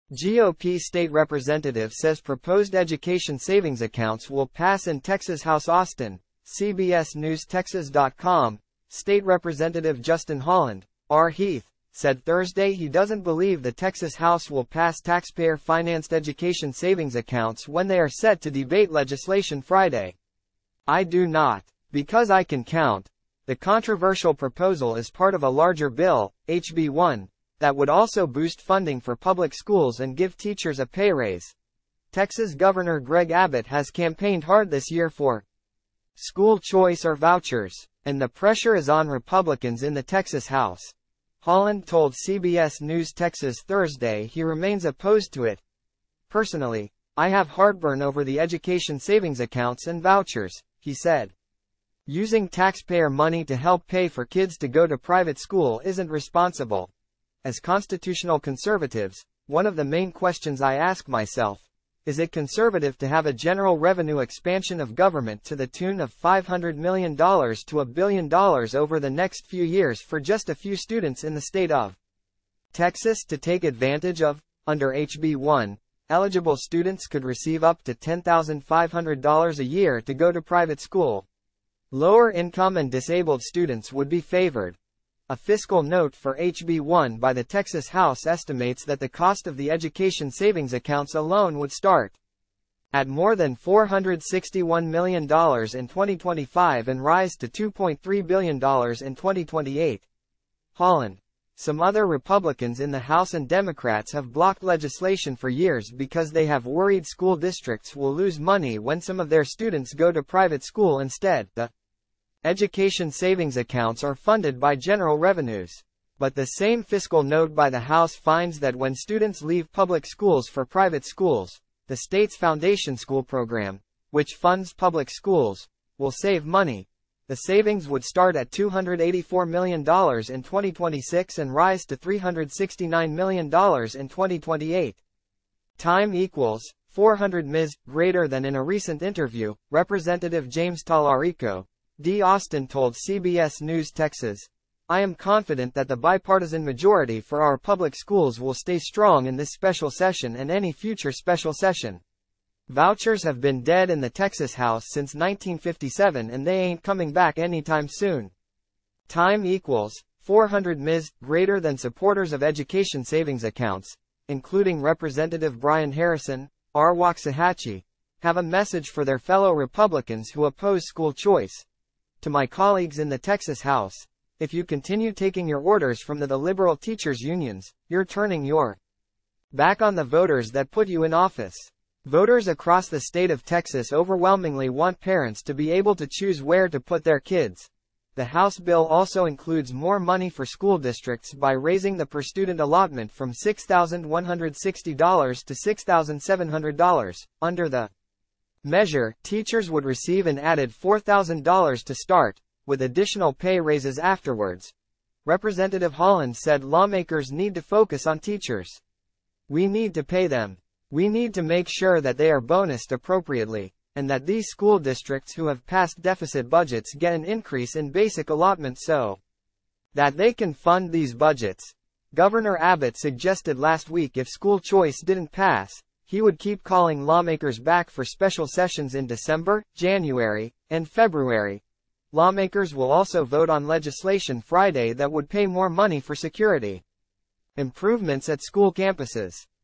GOP State Rep. says proposed education savings accounts will pass in Texas House
AUSTIN (CBSNewsTexas.com) - State Representative Justin Holland, R-Heath, said Thursday he doesn't believe the Texas House will pass taxpayer-financed education savings accounts when they are set to debate legislation Friday.

"I do not. Because I can count."

The controversial proposal is part of a larger bill, HB 1, that would also boost funding for public schools and give teachers a pay raise.

Texas Governor Greg Abbott has campaigned hard this year for school choice or vouchers. And the pressure is on Republicans in the Texas House.

Holland told CBS News Texas Thursday he remains opposed to it.

"Personally, I have heartburn over the education savings accounts and vouchers."

He said using taxpayer money to help pay for kids to go to private school isn't responsible.

"As Constitutional Conservatives, one of the main questions I ask myself, is it conservative to have a general revenue expansion of government to the tune of $500 million to a billion dollars over the next few years for just a few students in the State of Texas to take advantage of?"

Under HB 1, eligible students could receive up to $10,500 a year to go to private school. Lower income and disabled students would be favored.

A fiscal note for HB 1 by the Texas House estimates that the cost of the education savings accounts alone would start at more than $461 million in 2025 and rise to $2.3 billion in 2028.

Holland, some other Republicans in the House and Democrats have blocked legislation for years because they have worried school districts will lose money when some of their students go to private school instead.

The education savings accounts are funded by general revenues. But the same fiscal note by the House finds that when students leave public schools for private schools, the state's Foundation School Program, which funds public schools, will save money.

The savings would start at $284 million in 2026 and rise to $369 million in 2028.

In a recent interview, Representative James Talarico, D-Austin told CBS News Texas, "I am confident that the bipartisan majority for our public schools will stay strong in this special session and any future special session. Vouchers have been dead in the Texas House since 1957 and they aint coming back anytime soon."

Supporters of education savings accounts, including Representative Brian Harrison, R-Waxahachie, have a message for their fellow Republicans who oppose school choice.

"To my colleagues in the Texas House, if you continue taking your orders from the the liberal teachers unions, you're turning your back on the voters that put you in office. Voters across the state of Texas overwhelmingly want parents to be able to choose where to put their kids."

The House bill also includes more money for school districts by raising the per student allotment from $6,160 to $6,700.

Under the measure, teachers would receive an added $4,000 to start, with additional pay raises afterwards.

Representative Holland said lawmakers need to focus on teachers. "We need to pay them. We need to make sure that they are bonused appropriately, and that these school districts who have passed deficit budgets get an increase in basic allotment so that they can fund these budgets."

Governor Abbott suggested last week if school choice didn't pass, he would keep calling lawmakers back for special sessions in December, January, and February.

Lawmakers will also vote on legislation Friday that would pay more money for security improvements at school campuses.